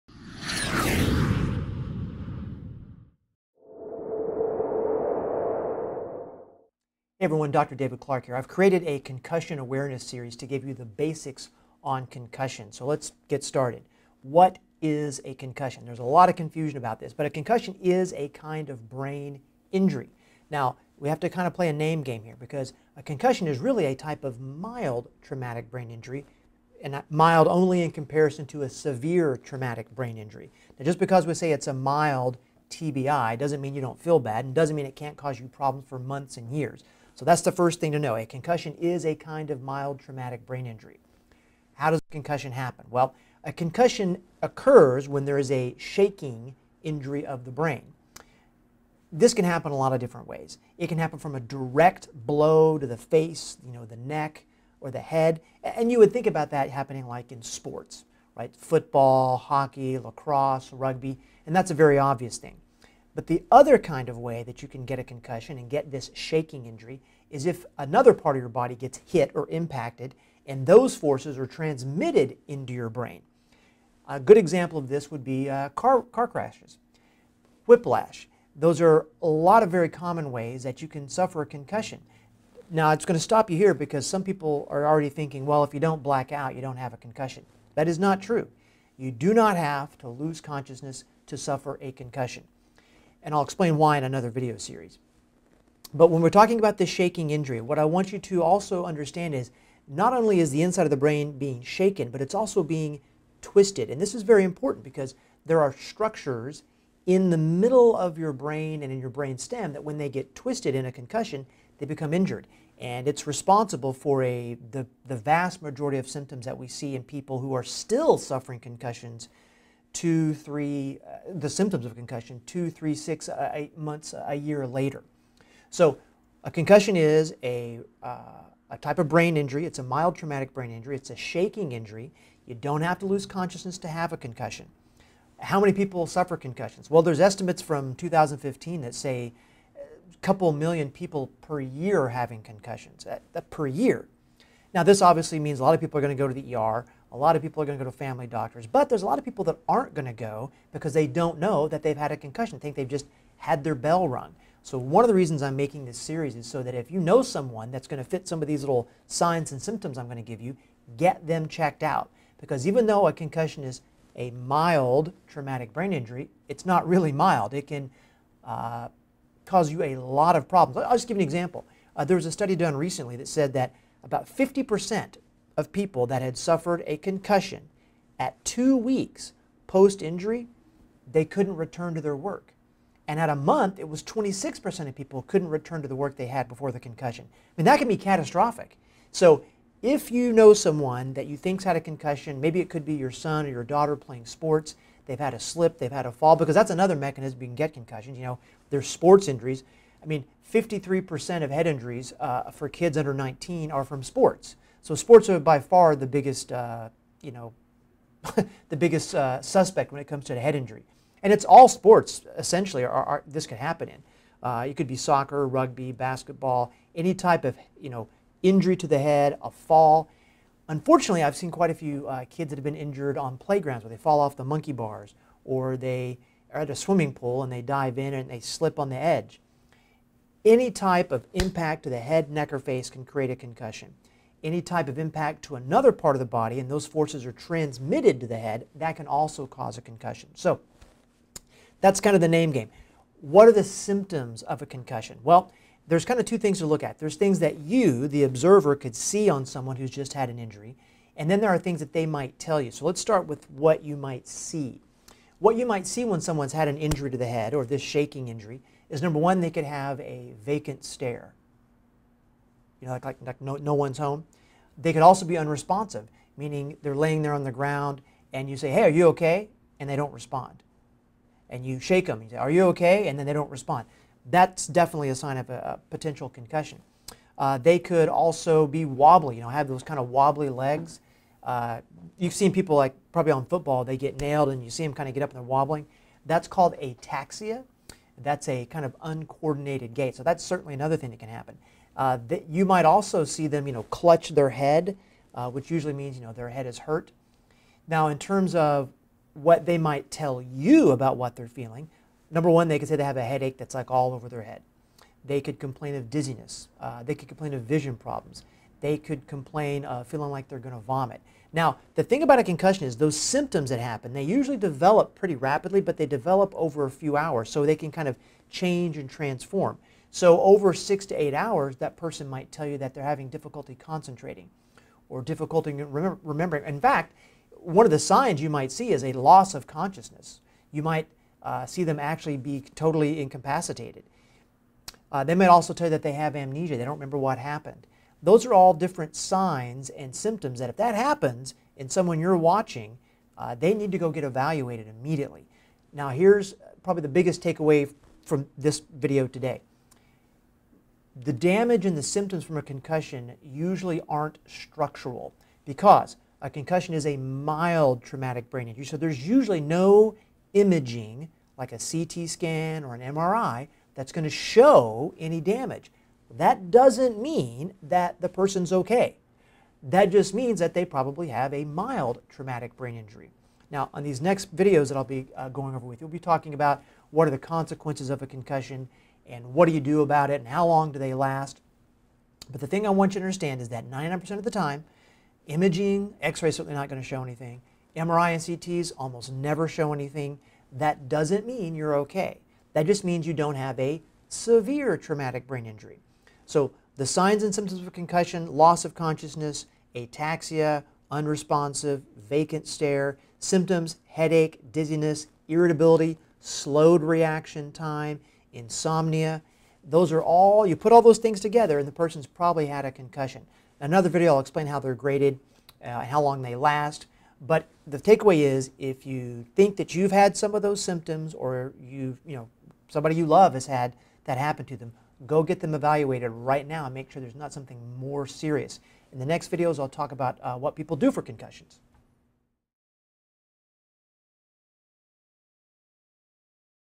Concussion Basics: "What is a Concussion?" "What are the symptoms of a Concussion?"
Dr. David Clark, DC helps you understand the basics of concussion: What is a concussion? What are the symptoms of concussion?

The most common symptoms of Post-Concussion Syndrome:
Fatigue
Dizziness
Vision problems
Headache
Trouble concentrating
Anxiety
Memory loss
Neck pain
Insomnia/sleep disturbance

Fellow American Association of Integrative Medicine

6015 Fayetteville Rd Ste 111